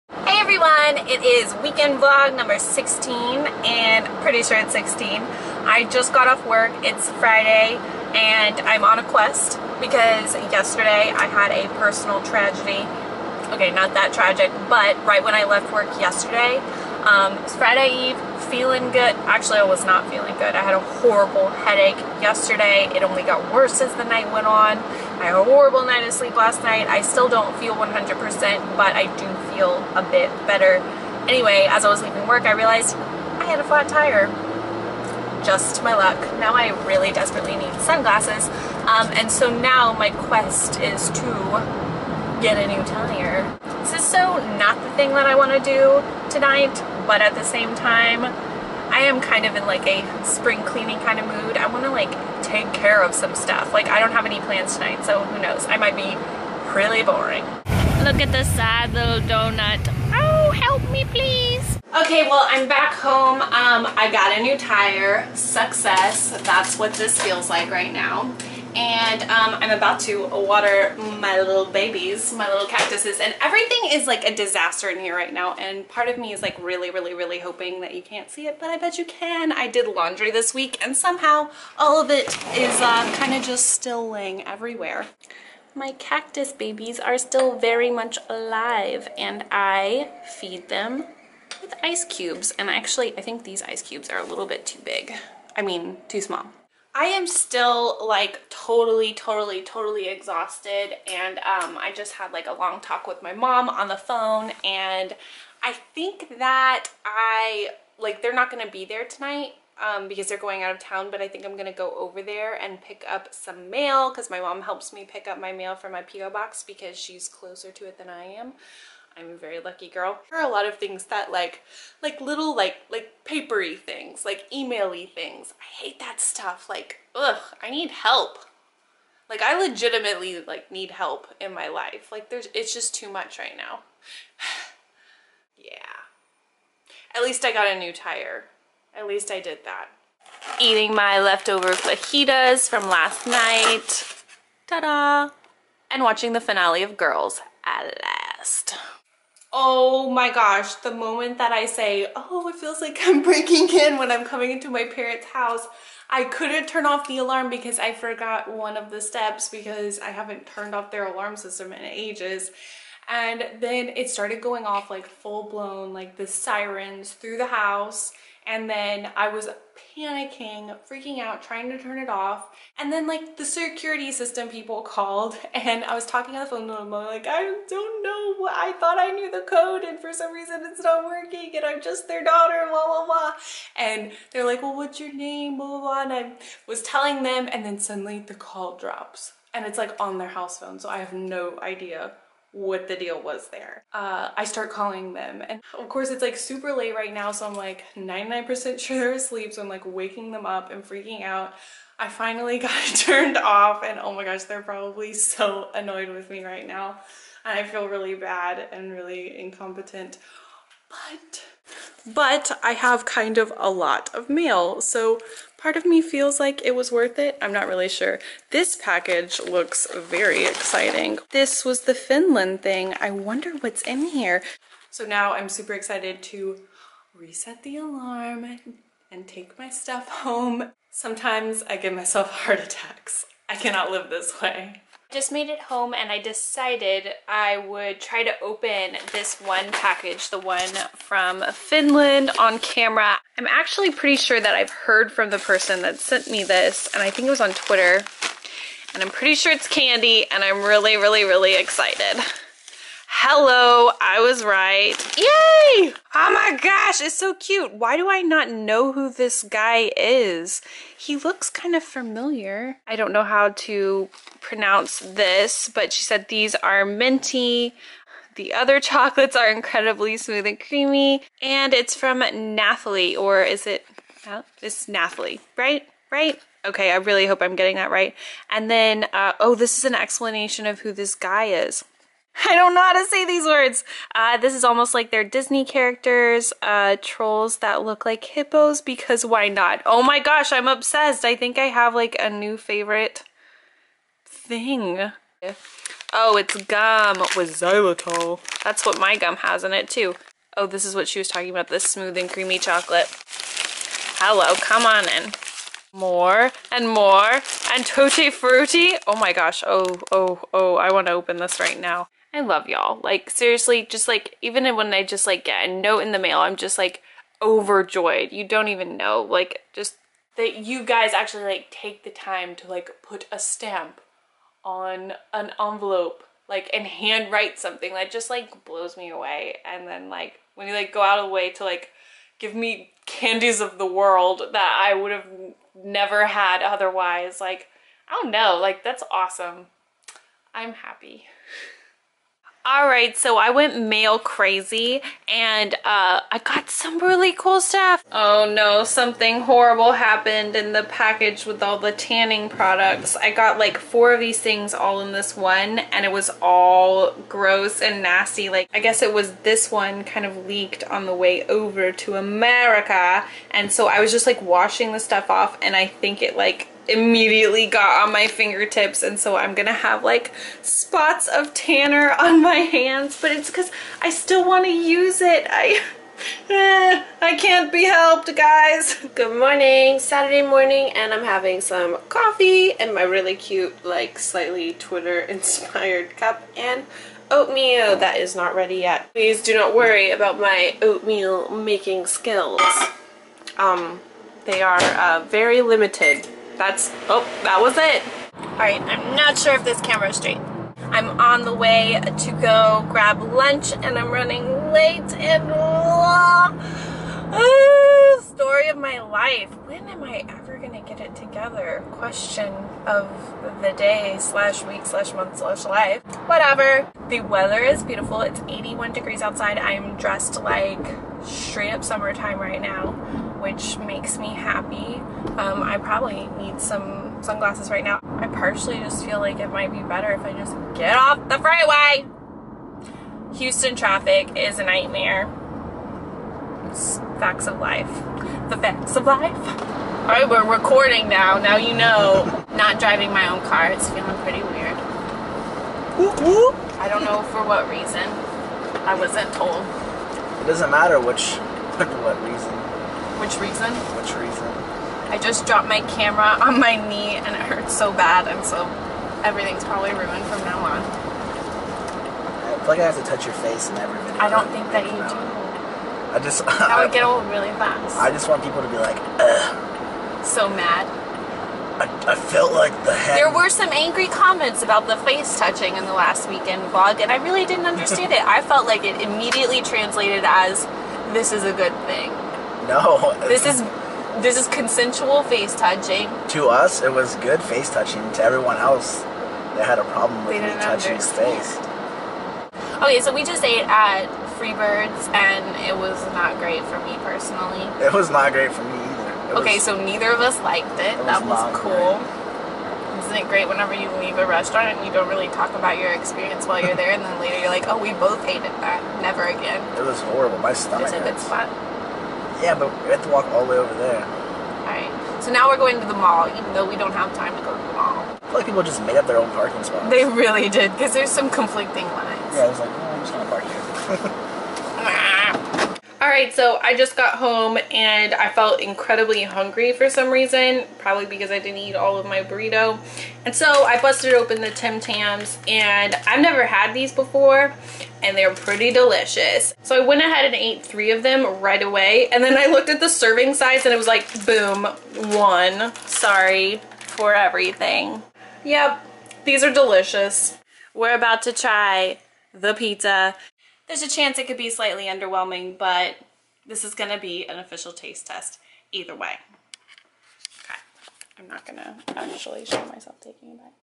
Candy of the World ♡ Weekend Vlog 16.1 | LeighAnnSays
Weekend Vlog 16 = Candy of the world mail party, a sad lil flat tire, BREAKING INTO MY PARENTS HOUSE, panicking about that, hating Freebirds, Tim Tam feast, and talking about consensual face touching.